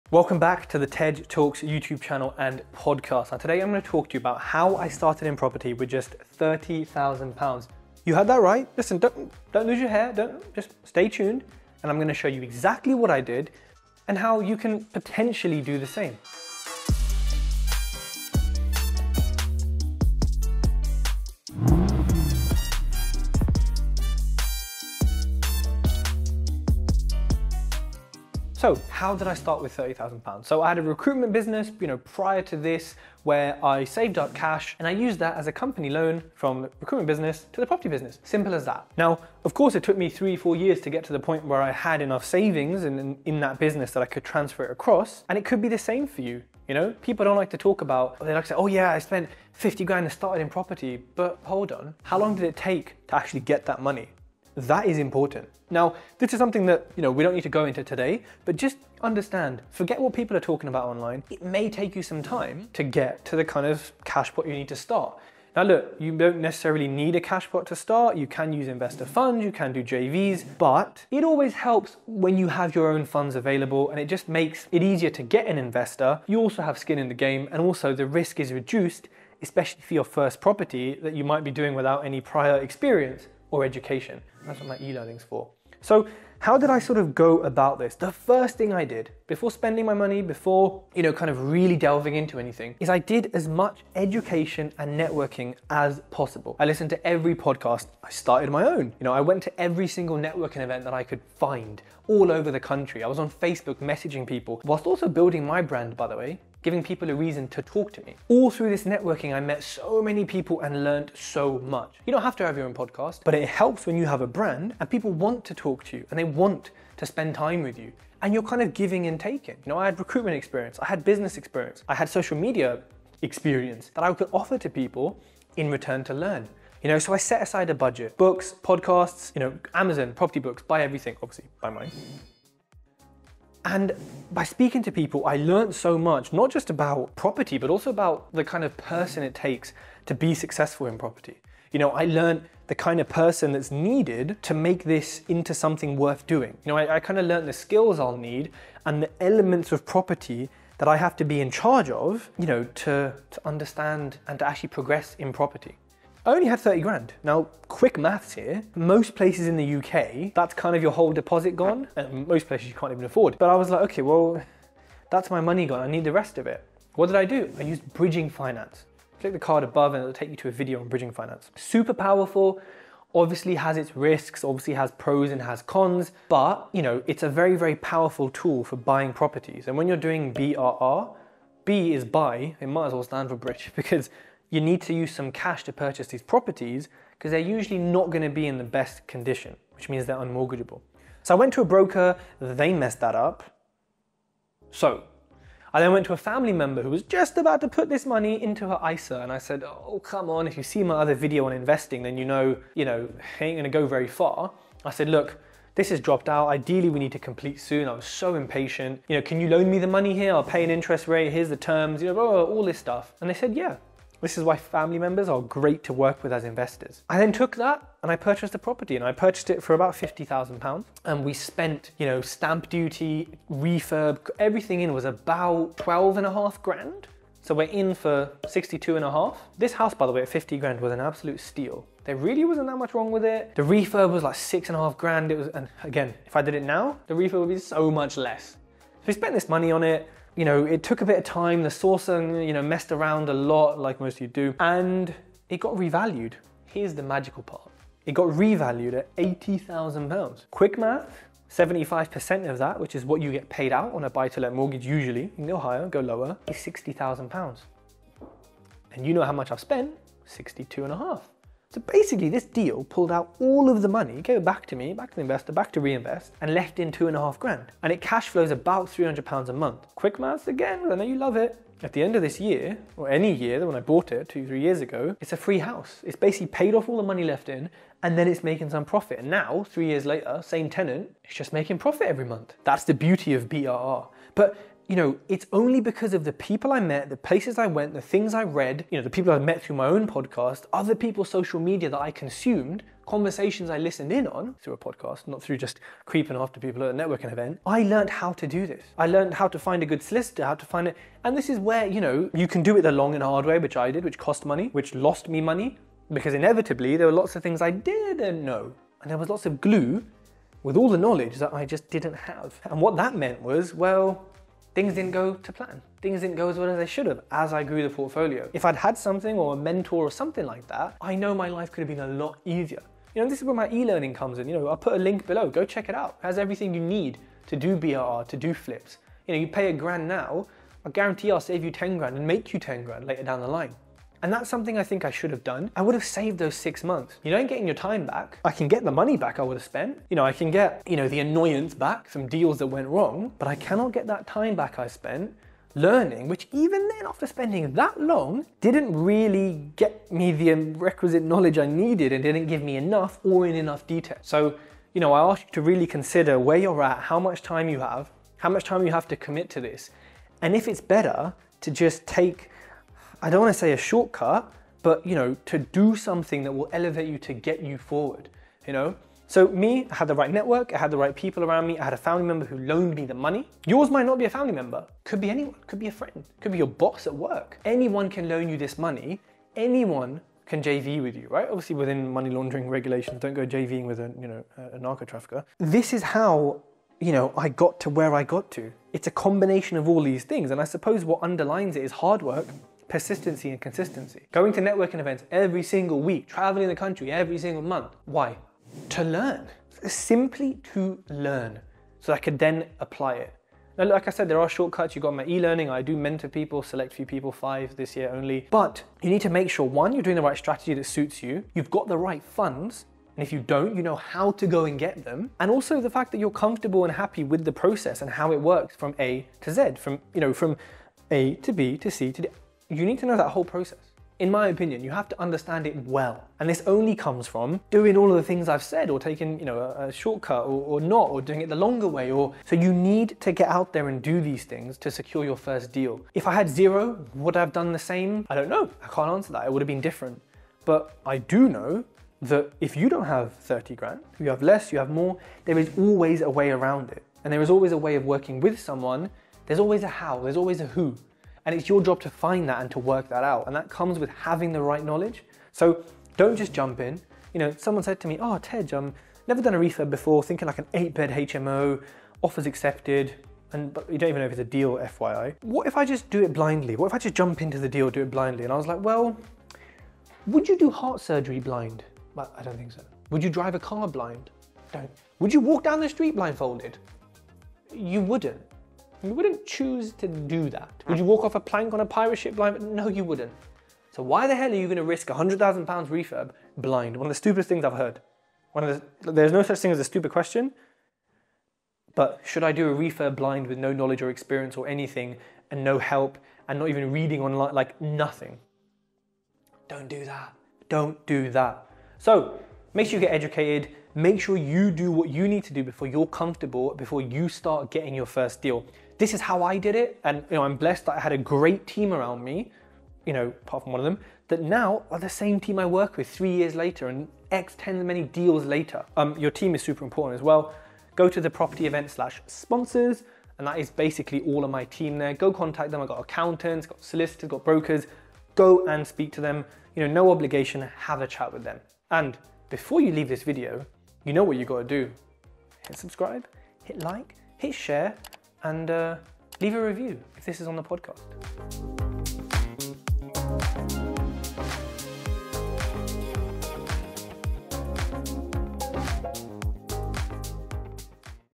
How I Turned £30k Into A Million Pound Property Portfolio
Have you ever wondered how much money you need to start in property?

This was also a question I had when I first started. So you're definitely not alone in asking that.

The truth is, you don't need that much to get started. I may have started with £30,000 in my bank account, but I quickly realised that the amount of money you've got is not really what matters.

Because, once I had paid the deposit on my first deal, I had nothing to pay off the refurb or the legal fees.

So what did I do to complete my first deal?

I went on a property course and got financially free within 2 hours...
Is probably what I would've done, had I listened to the countless gurus out there.

No, what I did was much more rooted in reality (and potentially boring?):

I used the education I had developed after hours of reading and watching podcasts. Once I knew enough, I was able to start raising finance through the steps you'll see in this video.

Stay till the end, and I'll show you exactly how I got started in property investing with £30,000 as well as how you can do the same with MUCH less.

You'll see the mistakes I made, what went wrong, and what you should do to avoid the same mistakes that I made early on.

My Podcast is available on ALL Podcast Players, including Spotify, Apple, Google & many more.

TIMESTAMPS

0:00 - Intro
0:29 - My Beginnings
1:25 - First Method
2:36 - First Deal
5:06 - Mistakes I Made
6:48 - What You Need To Do
7:36 - Don't Jump In Blindly
8:21 - Conclusion